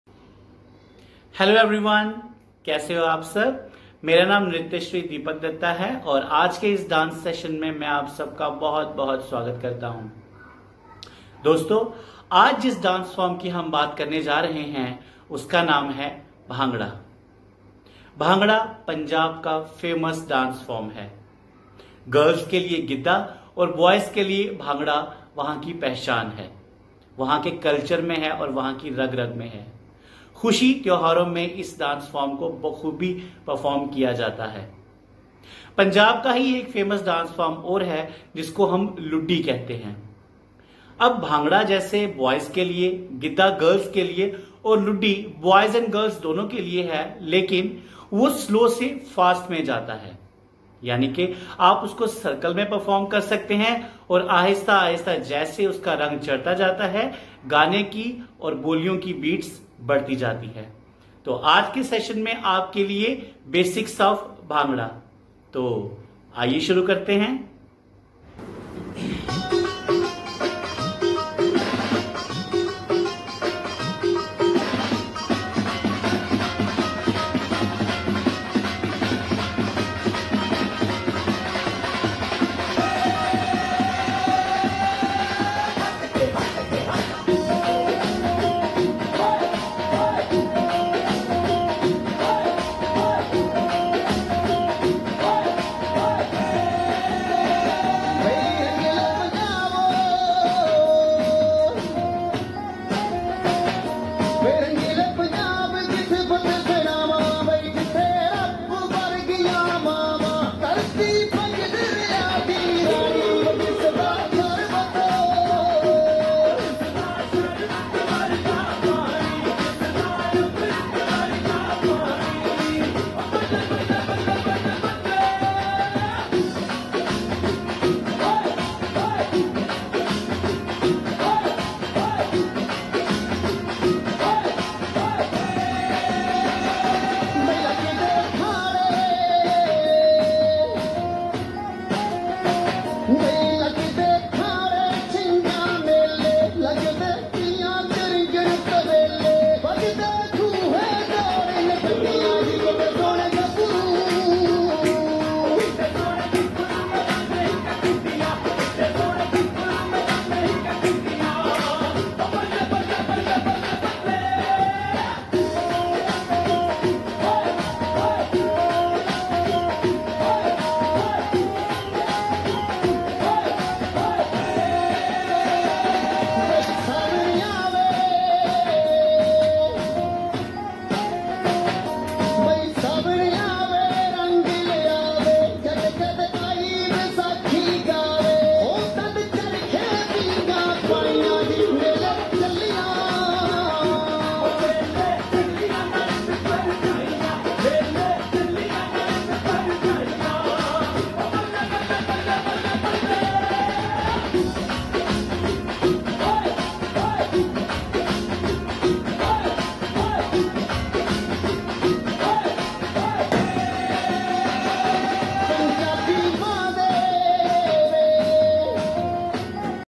Dance Class Session No.-18 Basics of Bhangra
Nrityashree Deepak Dutta, Dance Exponent Briefing about Bhangra and Performing Bhangra  with Basic steps for online session.

*BHANGRA*
        is an energetic, folk dance and music form that originated from Punjab, India. Today, it's the newest dance, music, and fitness phenomenon spreading all over the world. Bhangra has made its way to America's Got Talent, the London Olympics, and even the White House.
     It originated as a folk dance celebrated during the time of the harvest. Bhangra is traditionally danced to the dhol instrument, a large drum, and boliyan, short sets of lyrics that describe scenes or stories from Punjab. These lyrics most commonly reference themes of love, patriotism, strength, and celebration.
       Bhangra refers to several forms of folk dance and music that originate in the Punjab region of India. The dance is generally performed during the Vaisakhi festival that celebrates the harvest. Bhangra has a very energetic and lively tone and the dance is equally vivacious.
       *Bhangra dances differ from region to region and still retain their own unique identities. The term Bhangra has come to incorporate a whole host of these dance forms including Jhumar, Luddi, Giddha, Julli, Daankara, Dhamal, Saami, Kikli, and Gatka.*